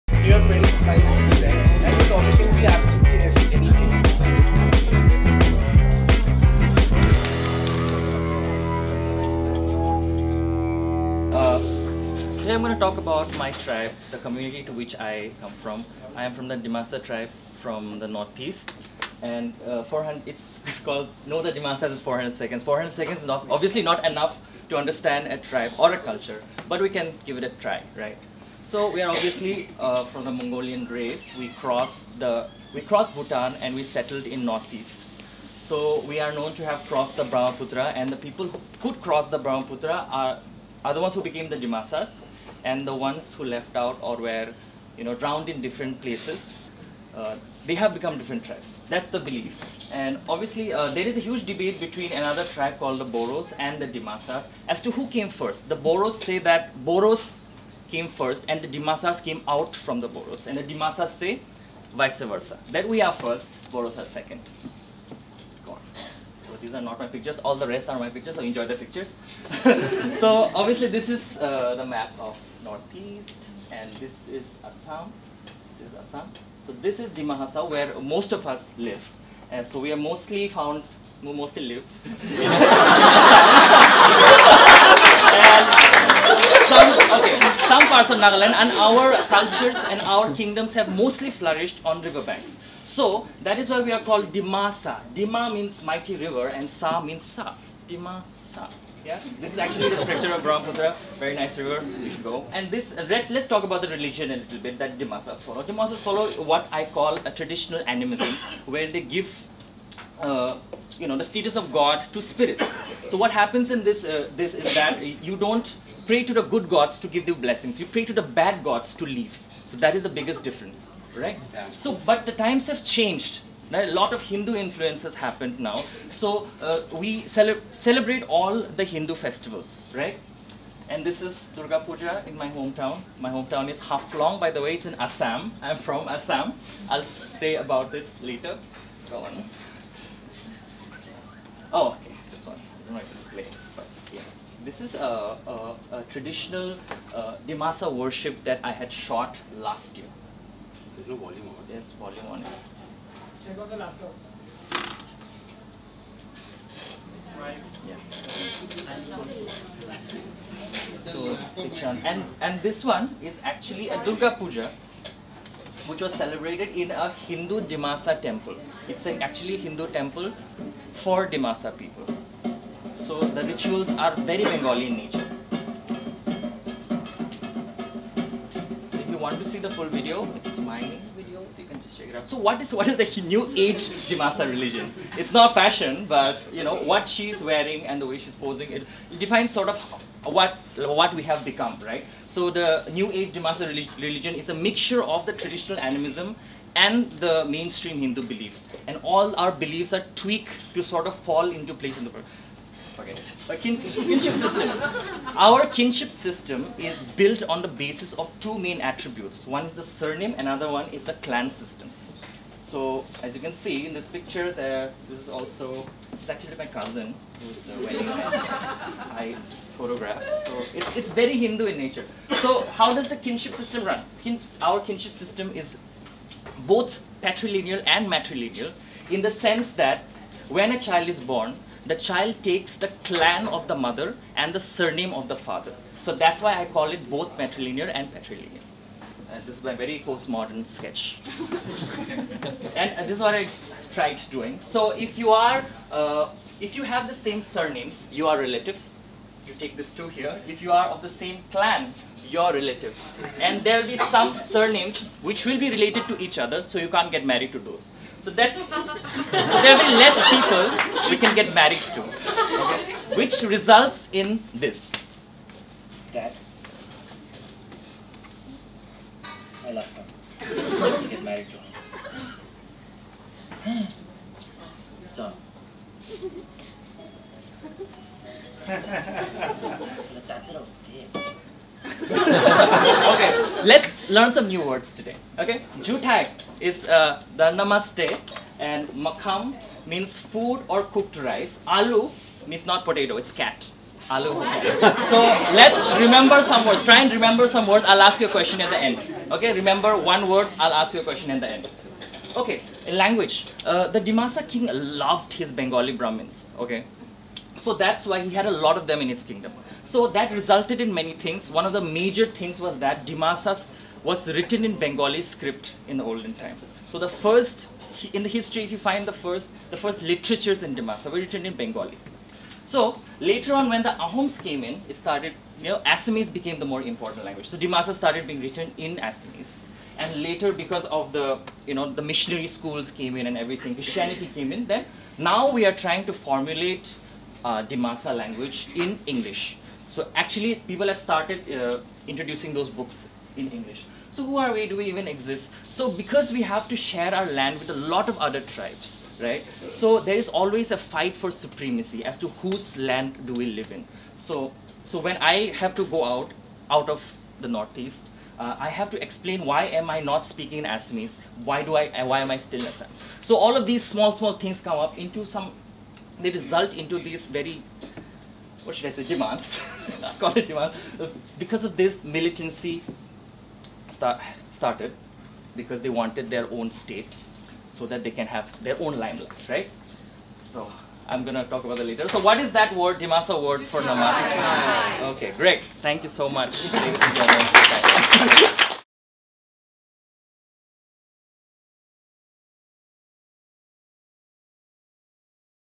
Know the Dimasas in 400 seconds | Vishal Langthasa | Pecha Kucha
Vishal talks about the Dimasa community and what makes them unique.

Pecha Kucha Fridays is a space for DY works employees to share their thoughts about things that matter to them.